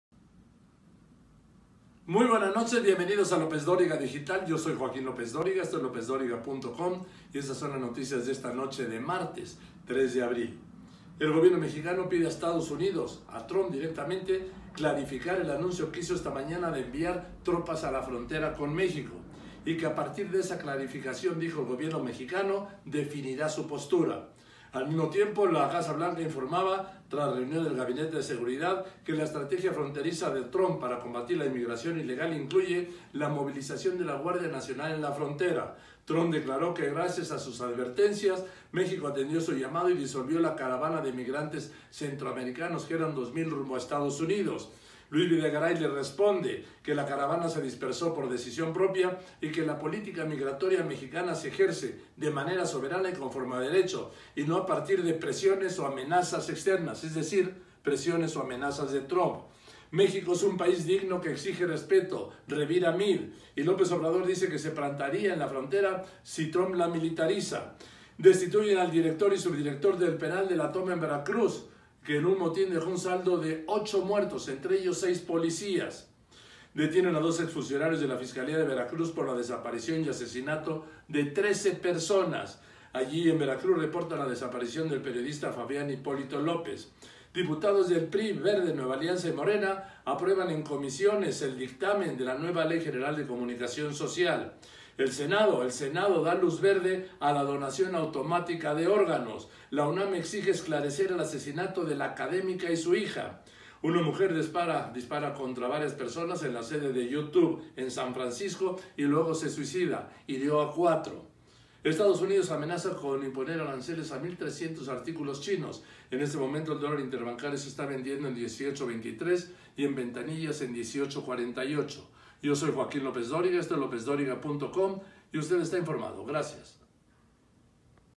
¡Las noticias! México pide a Trump clarificar anuncio de envío de tropas a la frontera
¡Las noticias! Gobierno de México pide a Trump clarificar anuncio de envío de tropas a la frontera; destituyen a director y subdirector de penal de La Toma y más